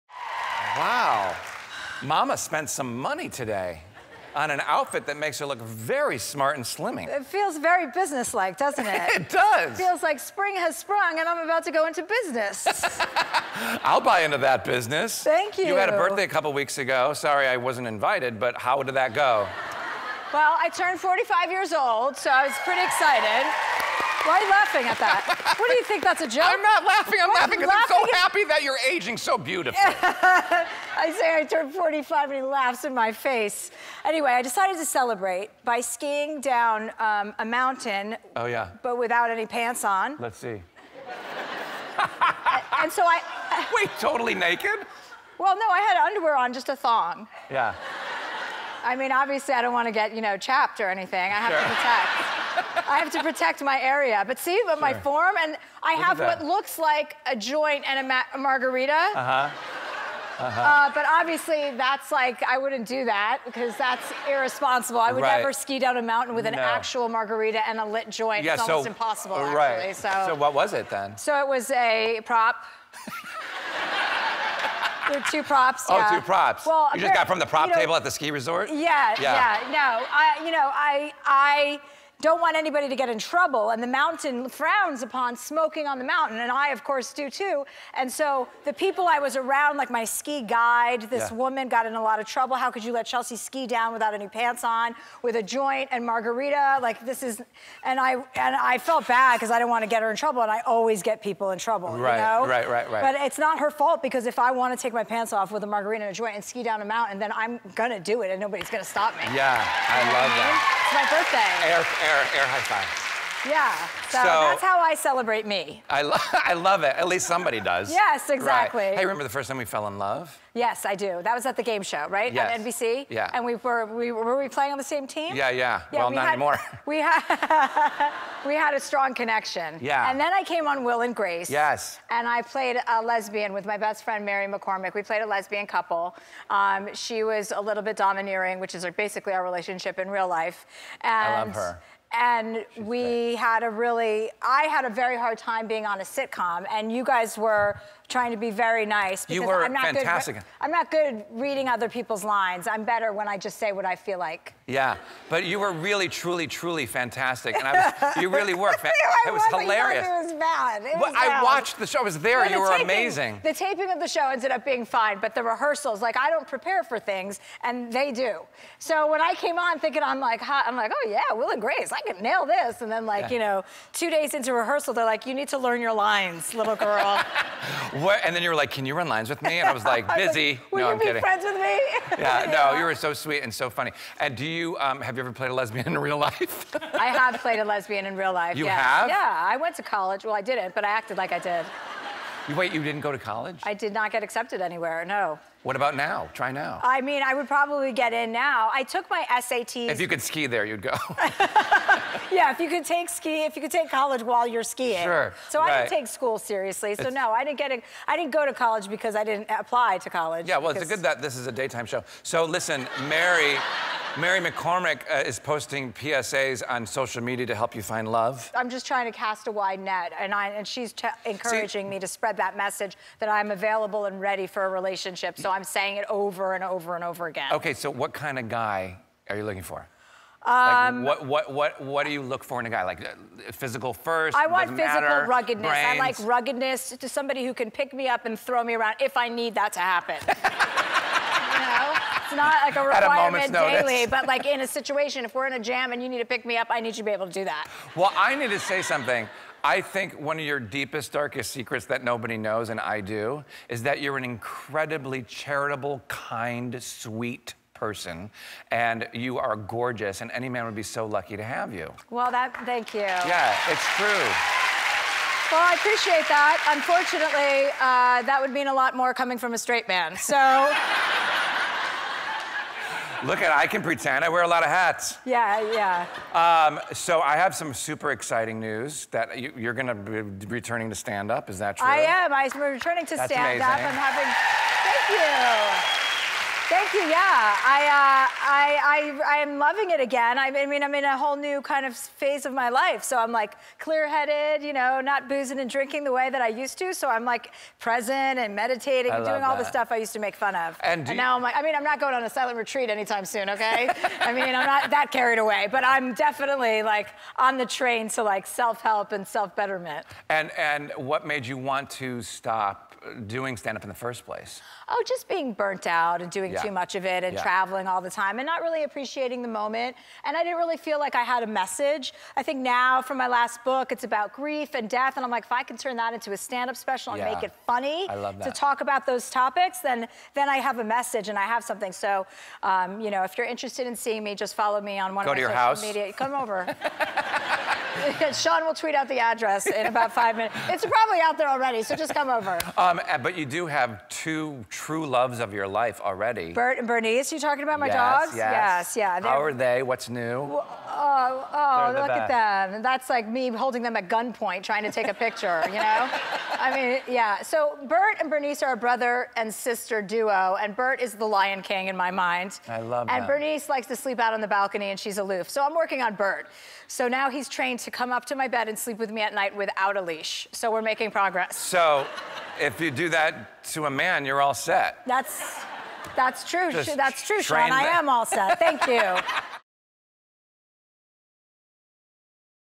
Chelsea Handler on Playing a Lesbian Couple with Her Real-Life Best Friend
Chelsea Handler talked with guest host Sean Hayes about her appearance on his show "Will & Grace," in which she played one-half of a lesbian couple with her real-life best friend, Mary McCormack. The comedian also chatted about spending her birthday skiing without pants, how she's single and ready to date, and her return to standup comedy.